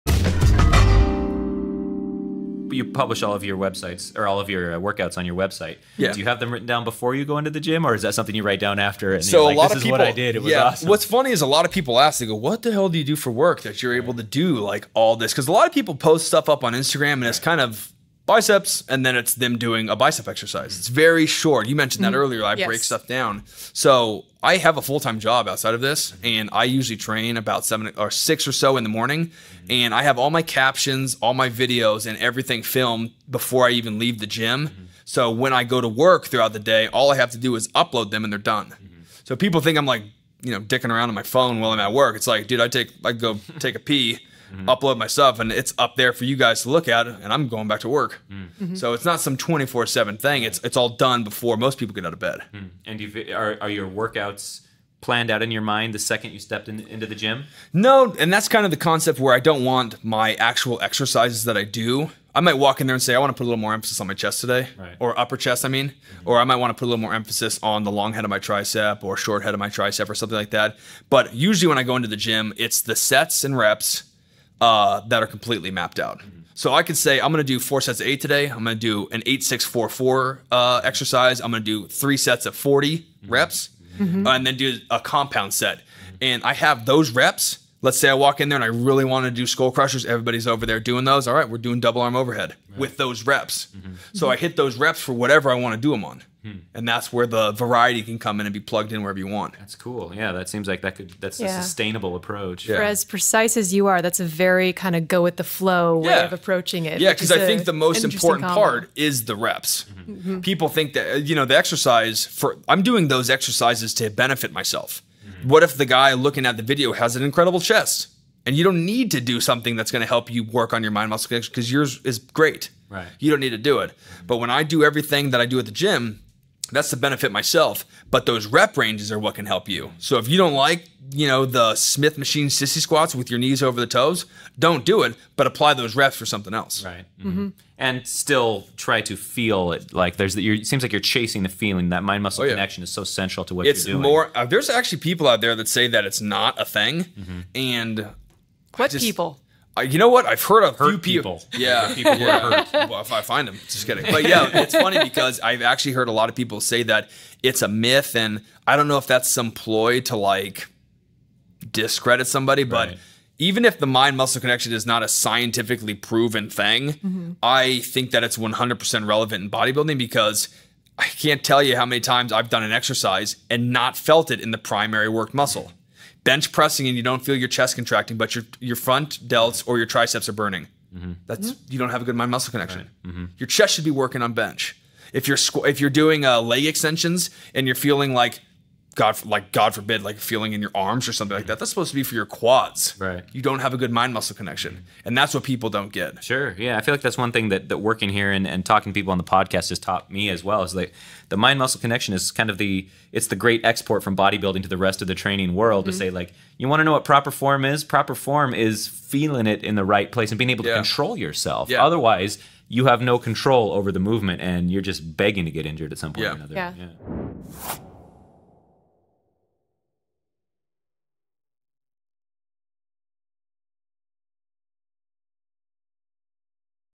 How to Train Intuitively | Julian Smith, The Quad Guy
Julian "The Quad Guy" Smith is known for creating innovative exercises and techniques. He walks into the gym with his mind open to whatever possibilities the available equipment allows.